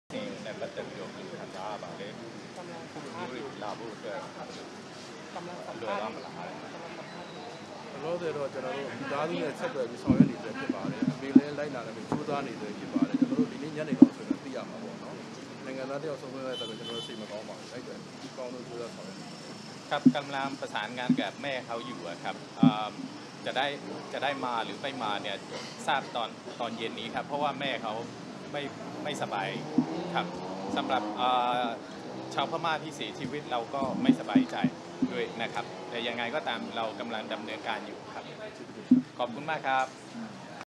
สถานทูตเมียนมาไม่สบายใจ เหตุกราดยิงพารากอนทำชาวเมียนมาดับ
รองเอกอัครราชทูตเมียนมา เผยสถานทูตรู้สึกไม่สบายใจ หลังชาวเมียนมาเสียชีวิตจากเหตุเยาวชนอายุ 14 ปี กราดยิงในห้างพารากอน